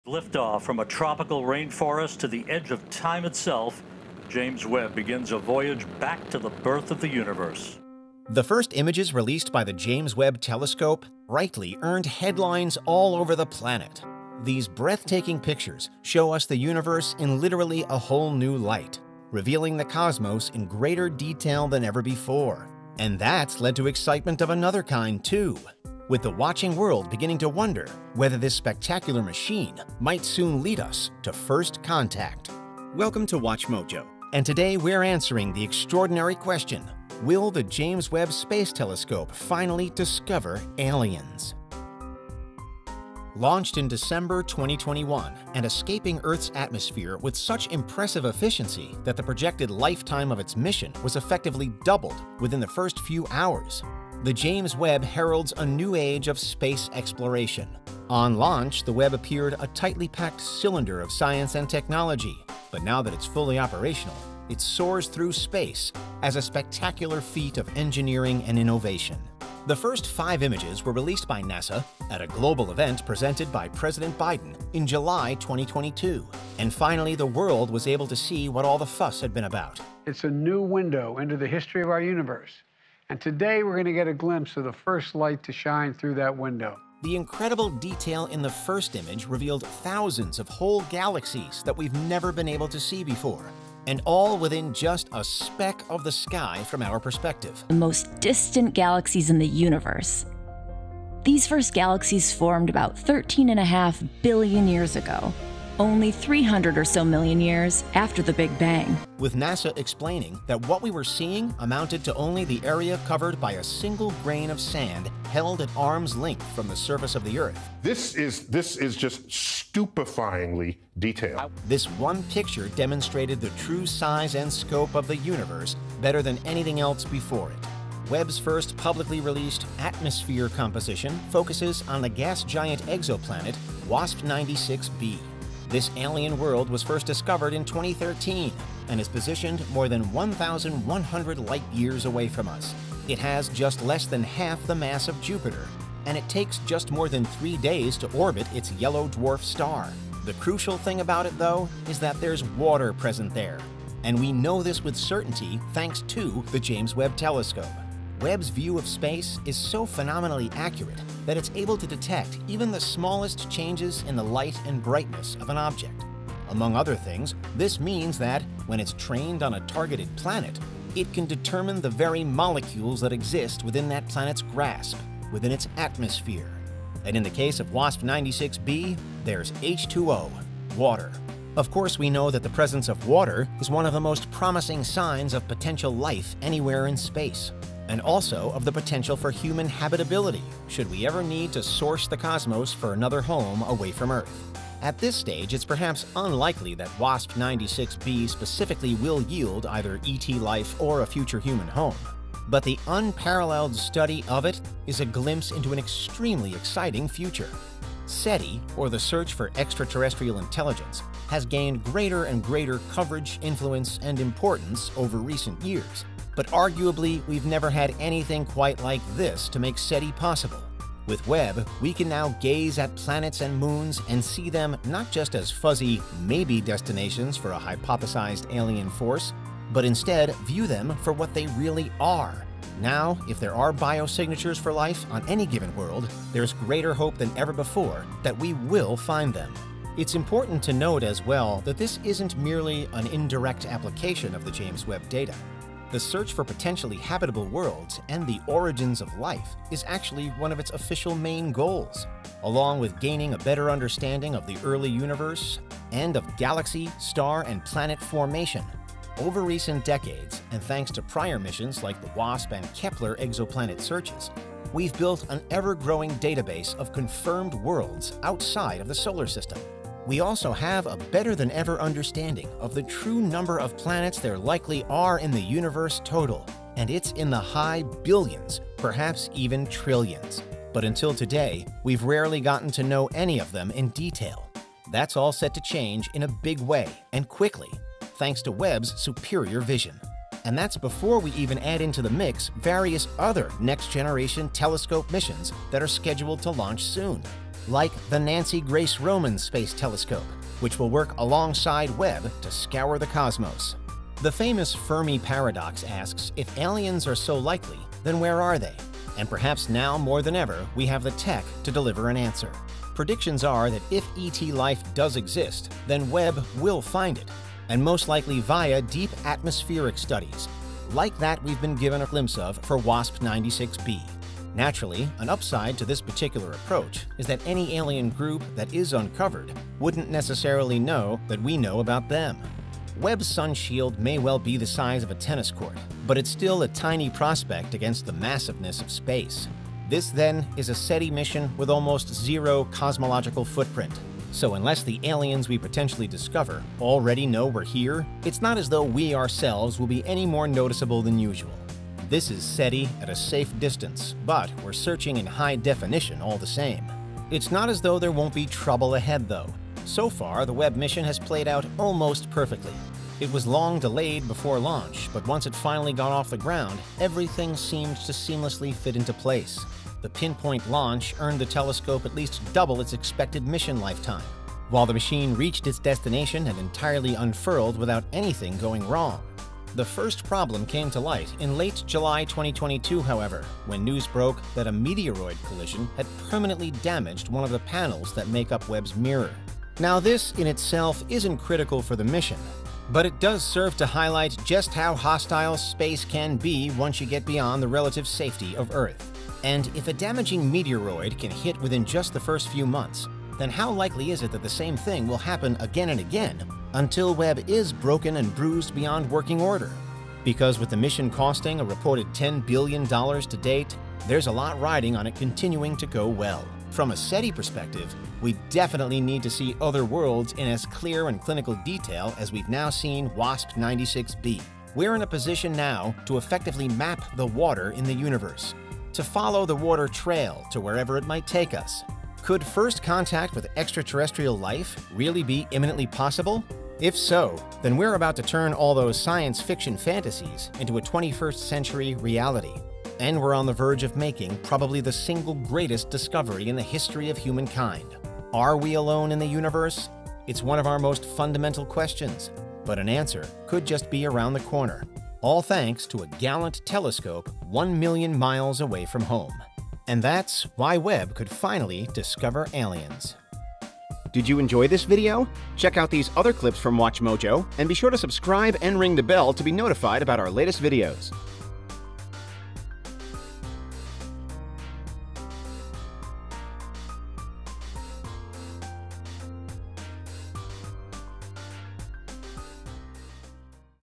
Will the James Webb Finally Discover Aliens?
In this video, WatchMojo takes a closer look at how the James Webb Space Telescope might finally discover alien life in the universe!

Are we about to discover alien life? Webb is looking into the universe in literally a whole new light, and that means that there will be so many new discoveries made... and the chances have never been higher that extraterrestrial life could be found!